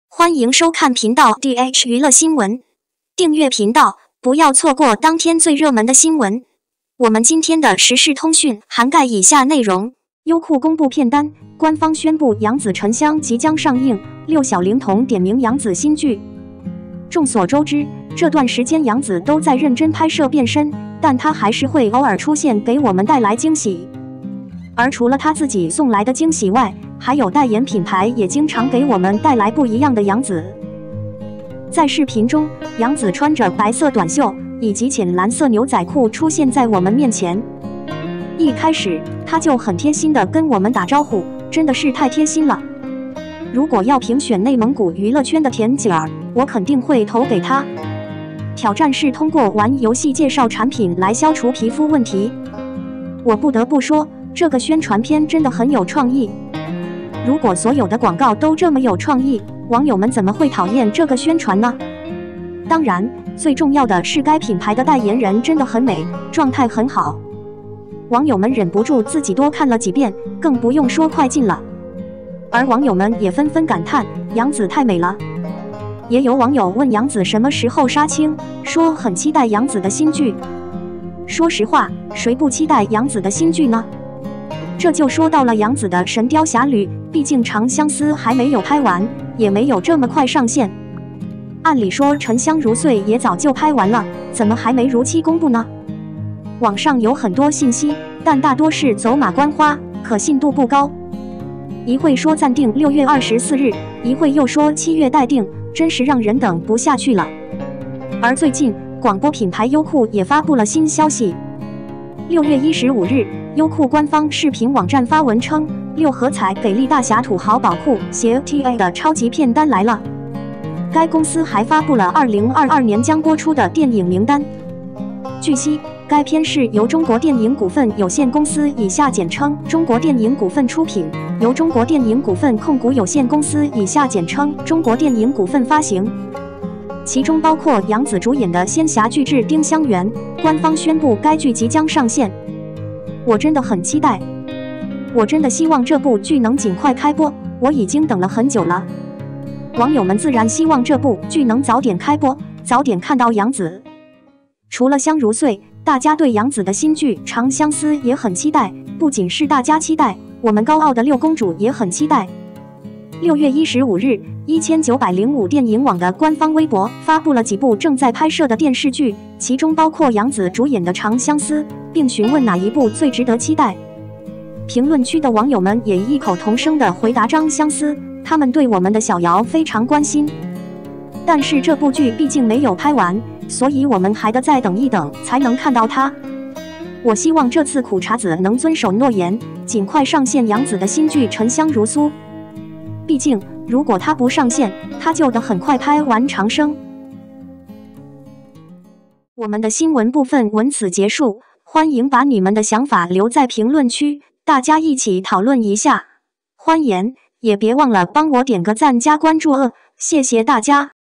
优酷公布，官宣杨紫《沉香》即将上线，六公主点名杨紫新剧！
优酷公布片单，官宣杨紫《沉香》即将上线，六公主点名杨紫新剧！
ENG SUB